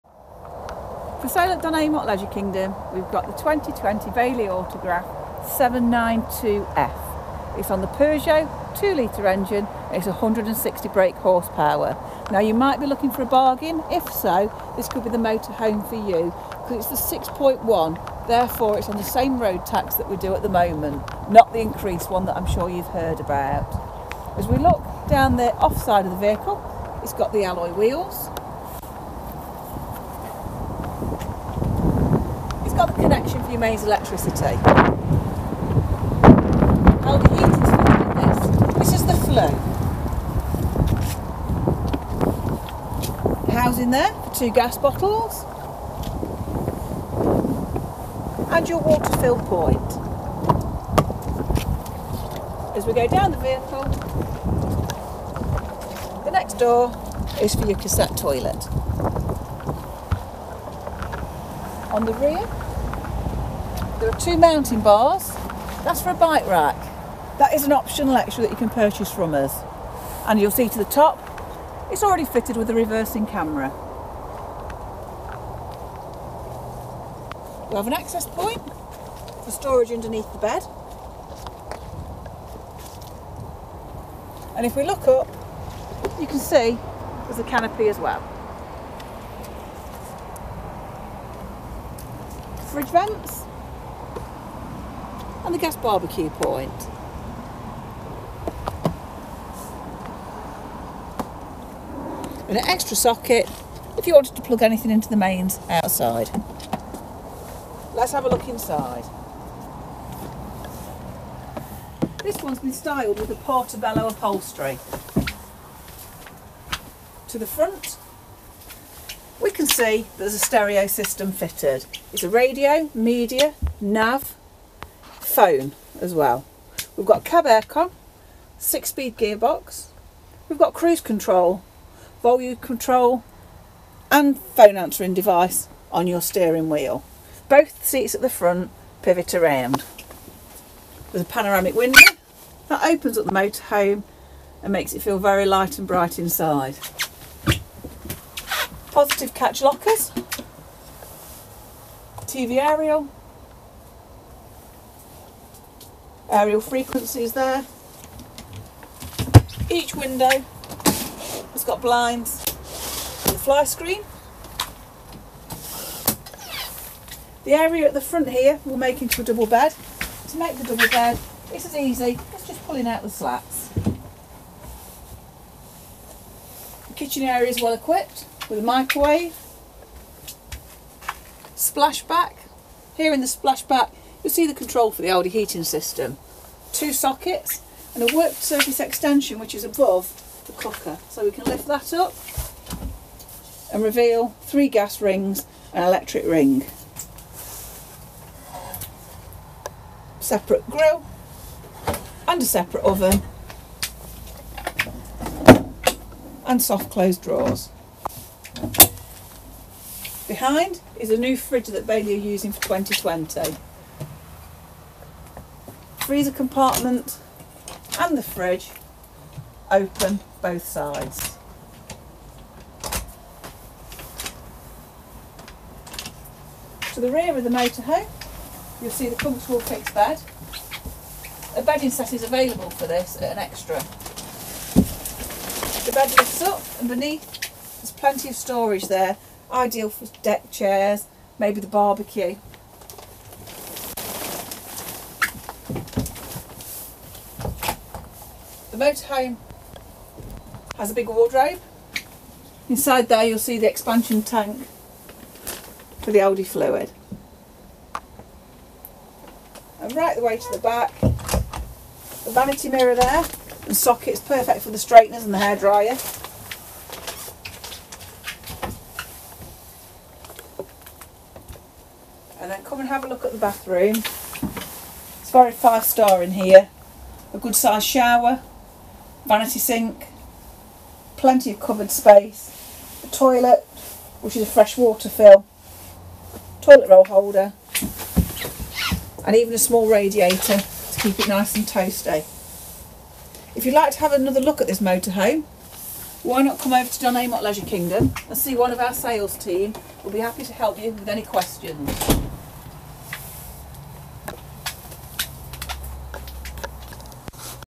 Bailey Autograph 79-2T
** BRAND NEW 2020 MODEL ** This Bailey Autograph 79-2 F is part of the newly refreshed range of Bailey Autographs. It has a smart graphite cab with colour-coded bumpers and the cab seats are now fully upholstered, another new feature is the flexible spotlight fitted above each of them, complete with a USB socket and a storage pocket nearby, perfect if you need to check your road atlas or a mobile phone.
This model is built on a Peugeot 6.1 Euro engine, 2.0 litre 160 bhp built to a 6-speed manual gearbox and this model has the 'Portobello' upholstery. This motorhome sleeps 4 and travels 4 and with a spacious lounge area that can transform to a double bed. The rear bedroom has a fixed french bed. The kitchen is to the nearside and comes fully equipped with 3-gas hob/hotplate, oven, grill, microwave oven and slimline fridge freezer opposite. There is a full width modern fitted washroom with shower and there is the comfort of the Alde central heating on board.
Externally we have a solar panel, barbecue point, status tv aerial, alloy wheels and roll-out awning.
Why not come along to Don Amott Leisure in order to view this brand new 2020 Bailey Autograph 79-2 F ?